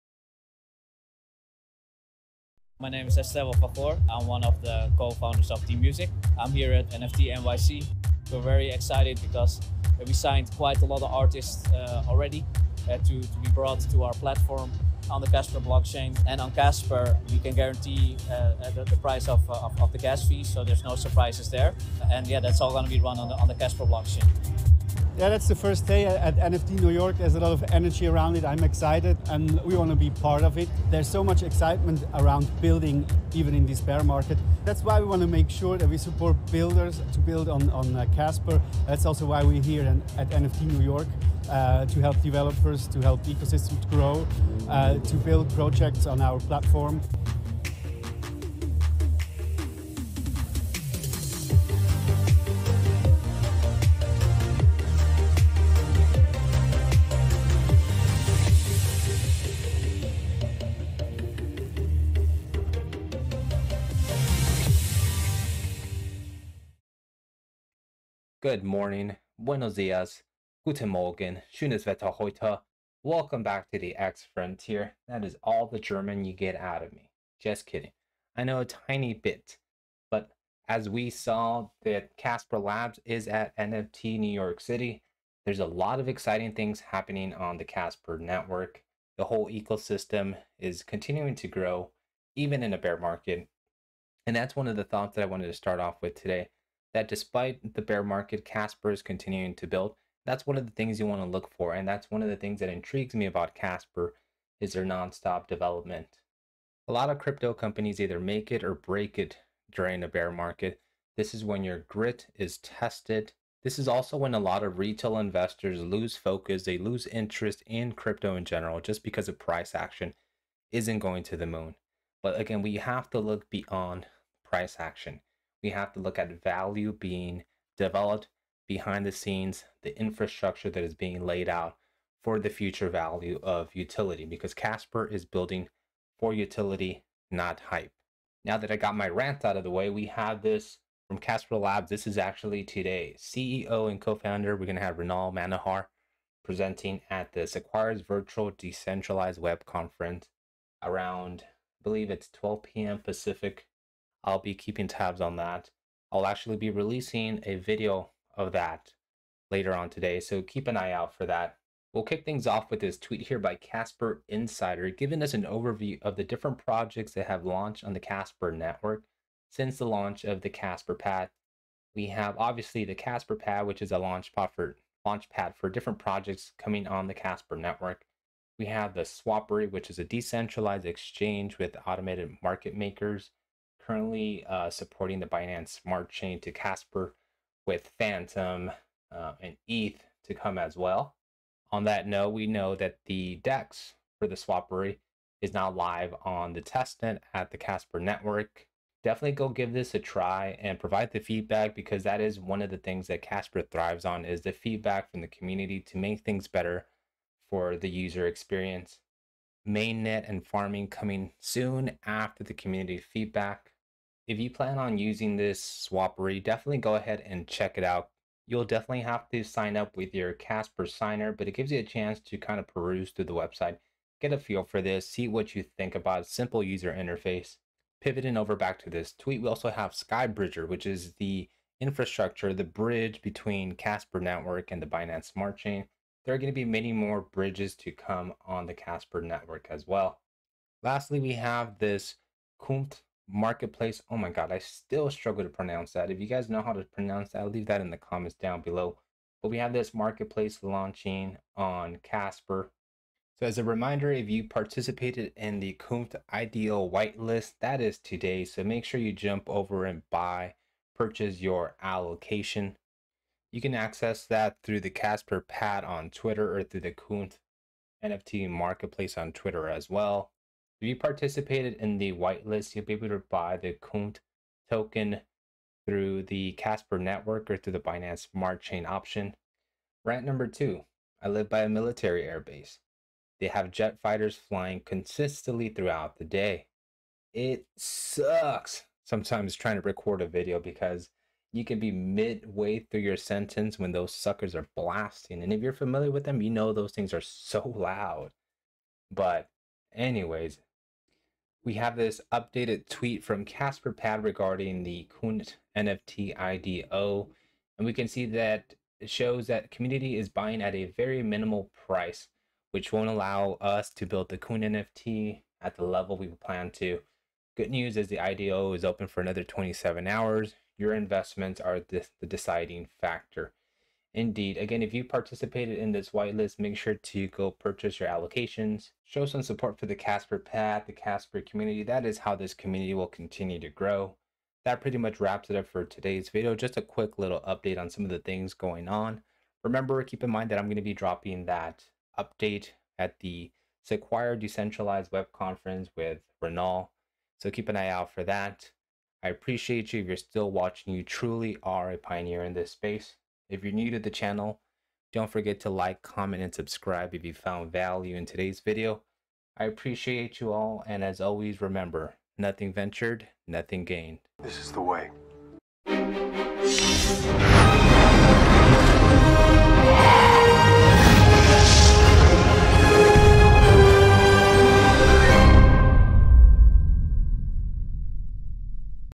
Casper Network NFT NYC | Project Updates & building utility
Welcome to the X-Frontier Channel.

In today’s short video we go over a few project updates on the Casper Network as well as taking a sneak peak of Casper at NFT NYC.